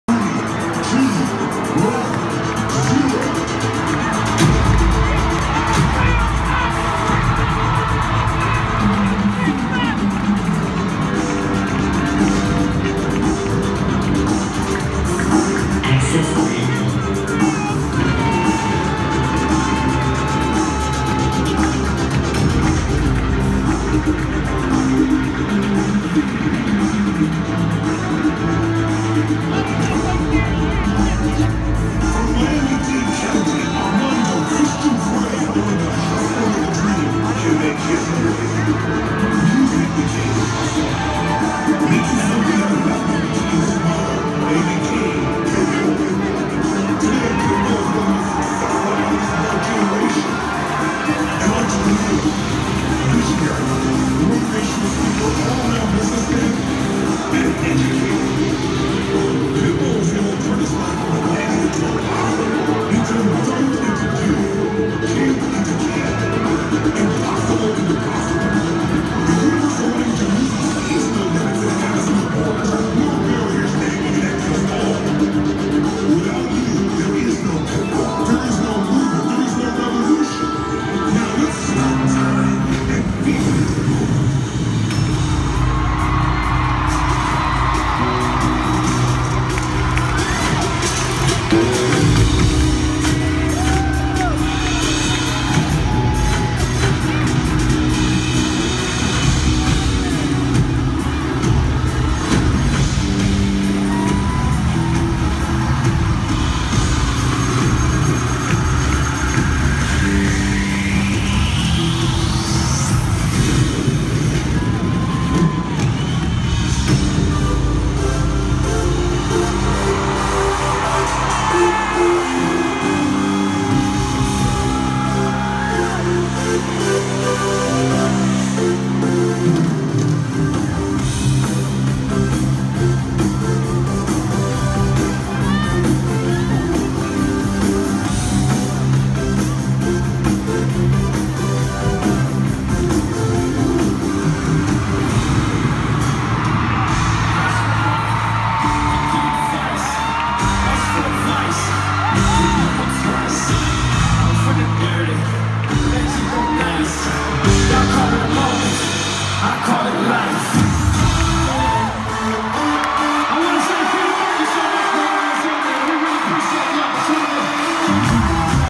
Pitbull Opener
Enrique Iglesias/Pitbull Tour on 10/13/17 in Philadelphia, PA